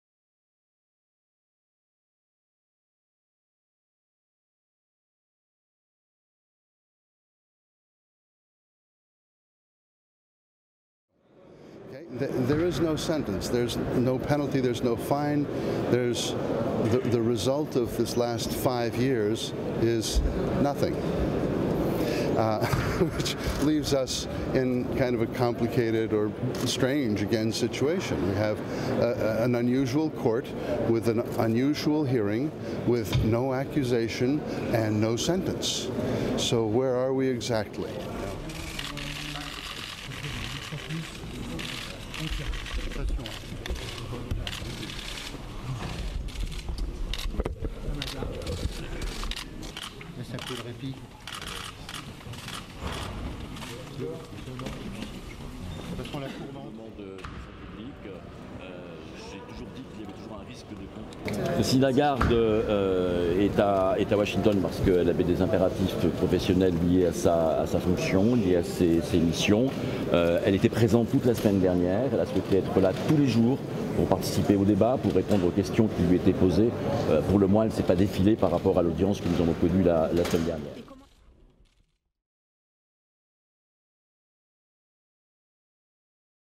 IMF Chief Christine Lagarde Found Guilty of Negligence
A French court Monday found Christine Lagarde guilty of negligence during her time as French finance minister but decided the International Monetary Fund head should not face any penalty. Photo: AP.